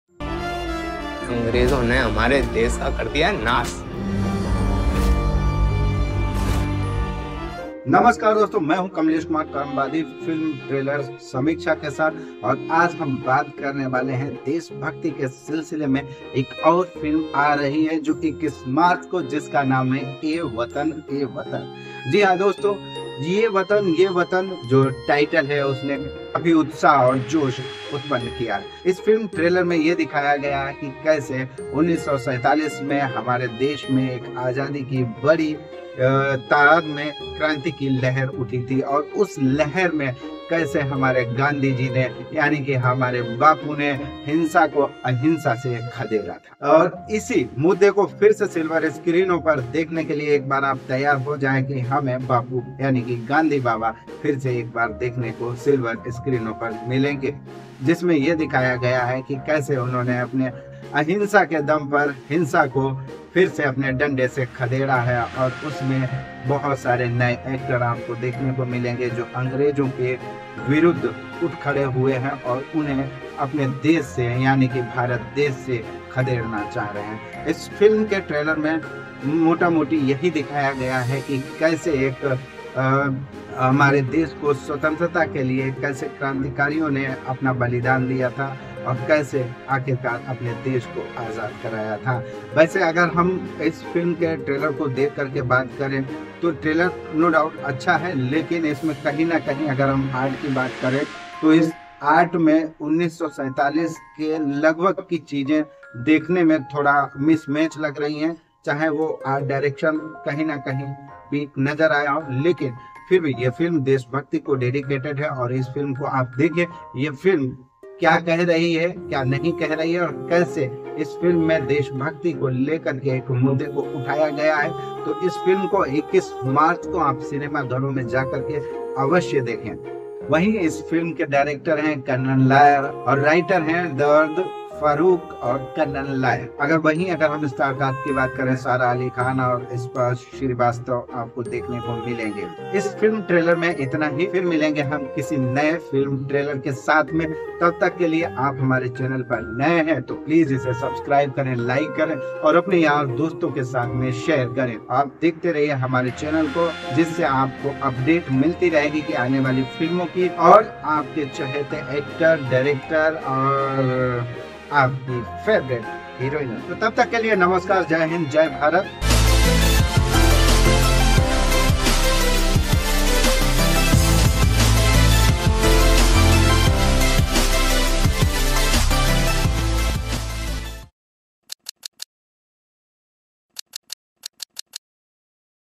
AE WATAN MERE| TRAILER REVIEW| SARA ALI KHAN| SPARSH SHRIVASTAV| KAMLESH KUMAR KARMVADI
Ae Watan Mere Watan is a historic thriller-drama. A Dharmatic Entertainment production, the movie is produced by Karan Johar, Apoorva Mehta and Somen Mishra. Directed by Kannan Iyer, the film has been written by Kannan Iyer and Darab Farooqui and features actor Sara Ali Khan in the lead alongside Sachin Khedekar, Abhay Verma, Sparsh Shrivastav, Alexx O' Nell and Anand Tiwari in pivotal roles and a special guest appearance by Emraan Hashmi